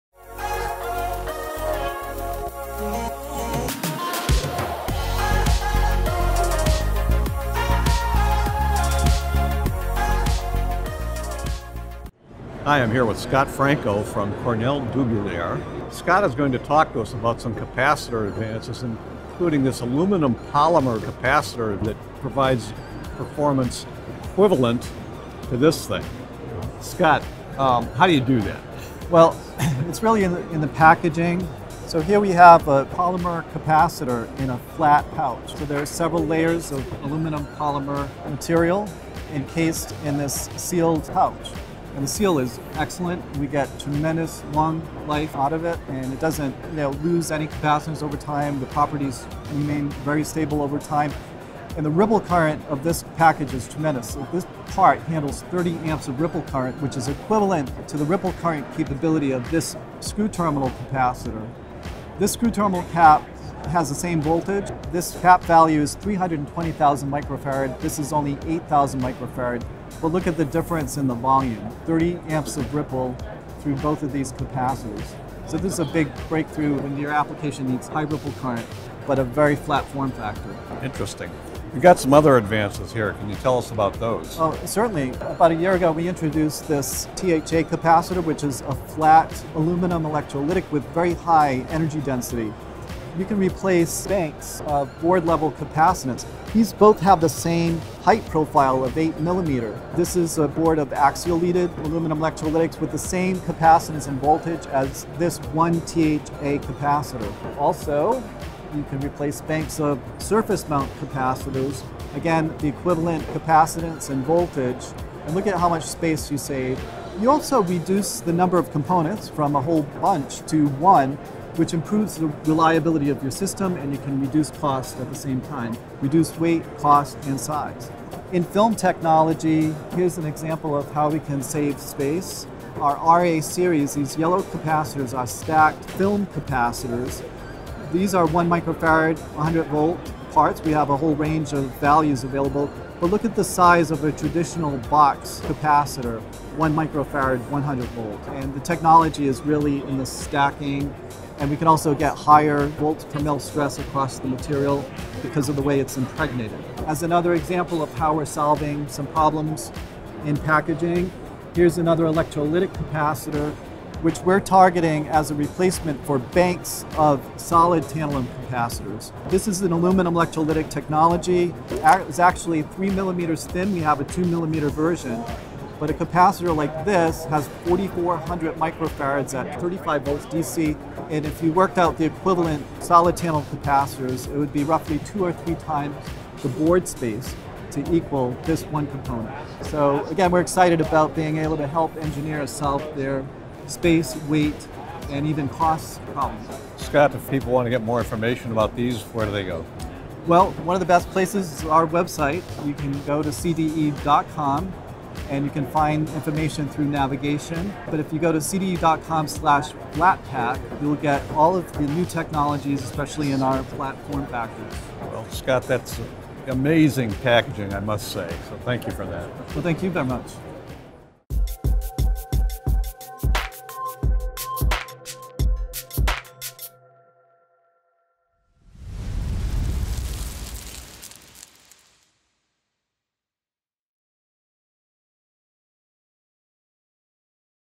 APEC 2019: Cornell Dubilier - Big capacitance for small spaces
Scott Franco of Cornell Dubilier explains the advances that are making it possible to package capacitors in much more compact packages, providing capacities in volumes that are two to three times smaller than what was possible just a few years ago. Examples include an innovative aluminum-polymer pouch battery that handles 30 A or ripple current, low-profile aluminum-electrolytic caps that sit just 8 mm high, and film capacitors that are just 3 mm high.